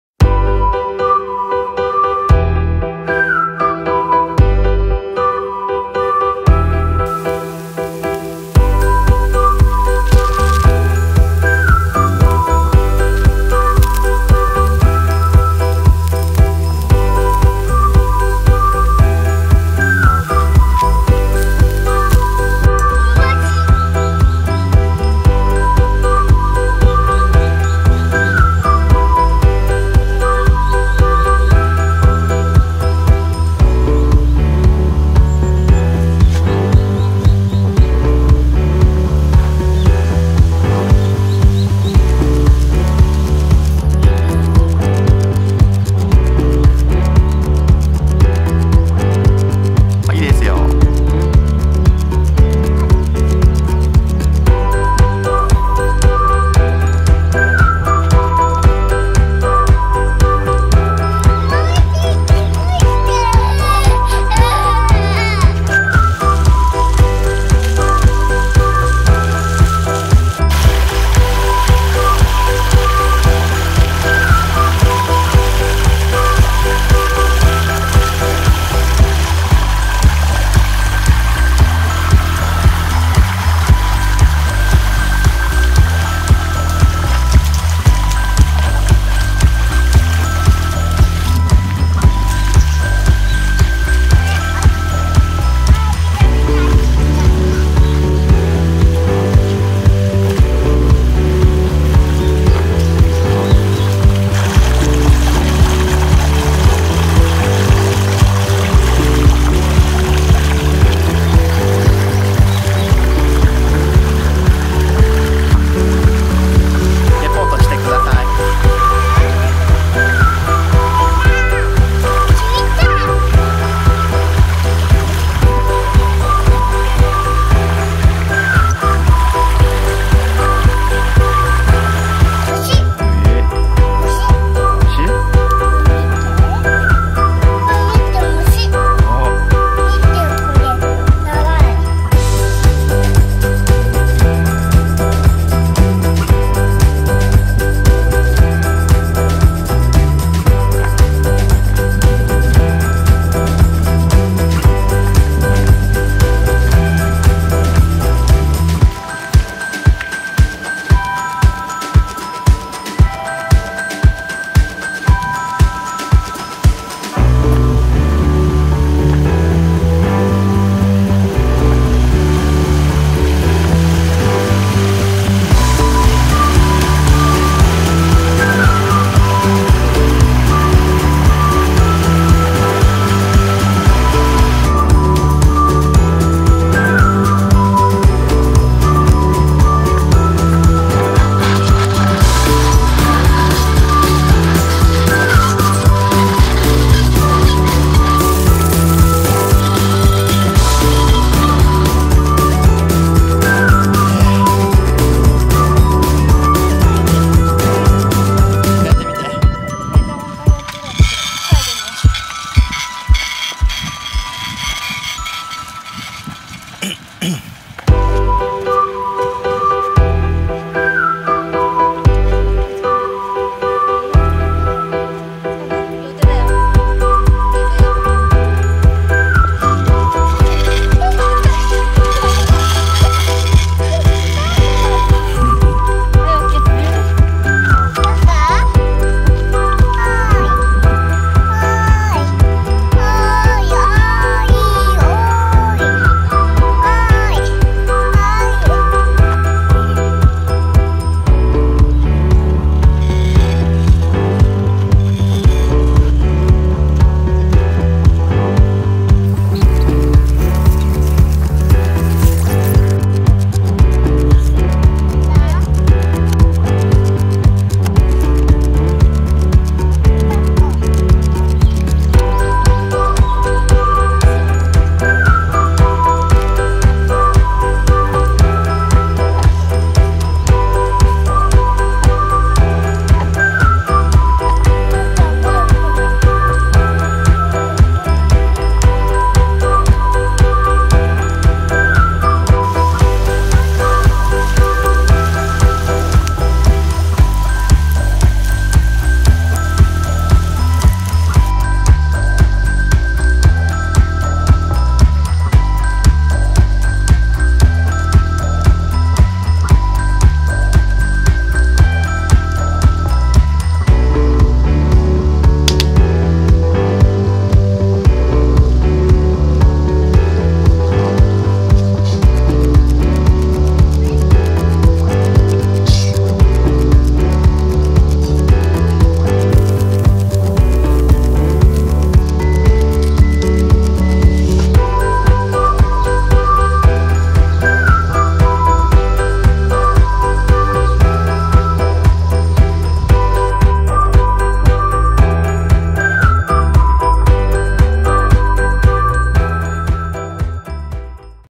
キャンプVO.1
青川峡キャンピングパークに行ってきました！